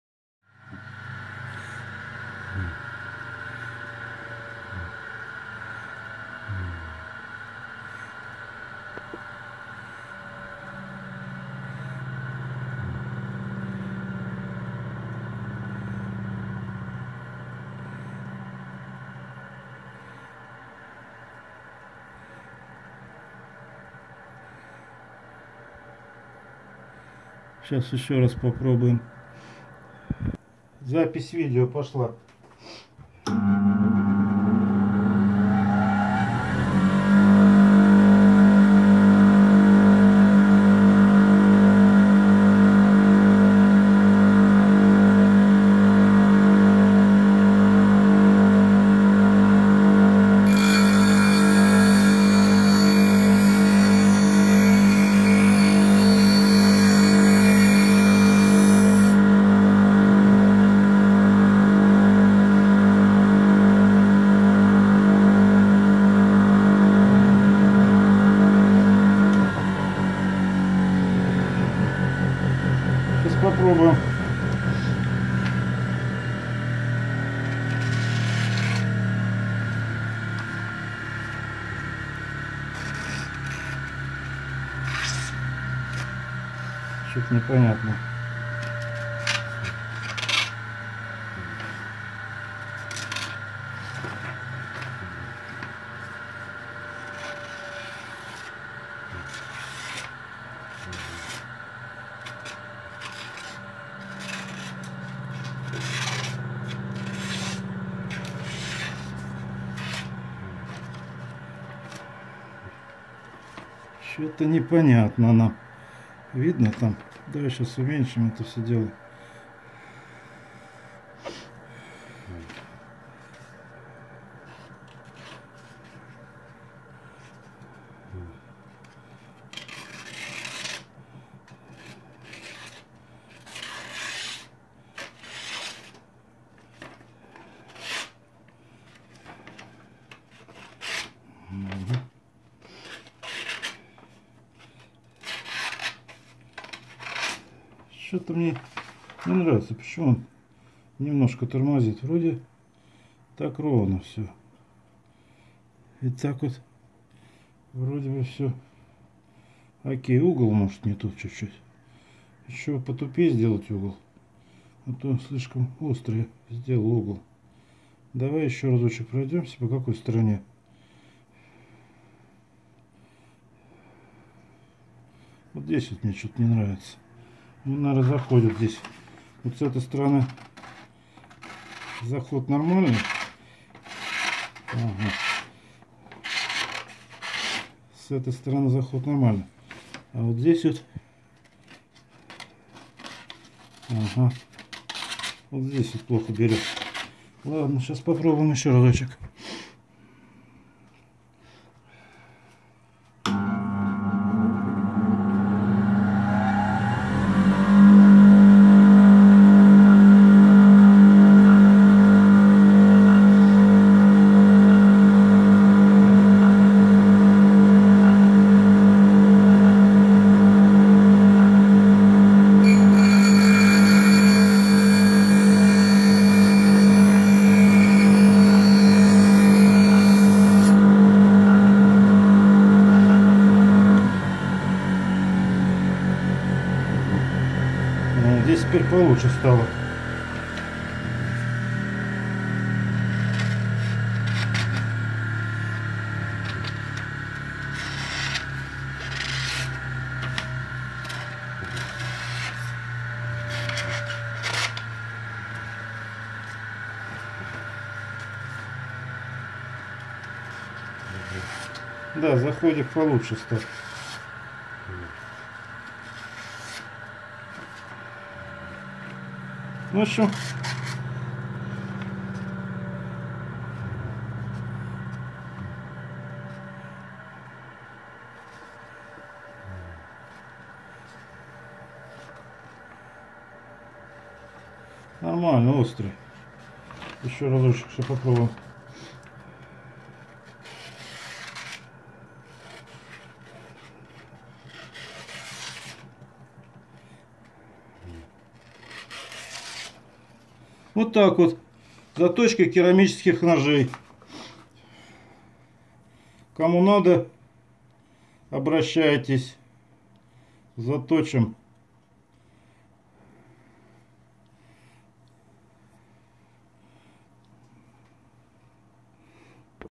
Затачиваем керамический нож.№71
На алмазном круге можно заточить керамический нож.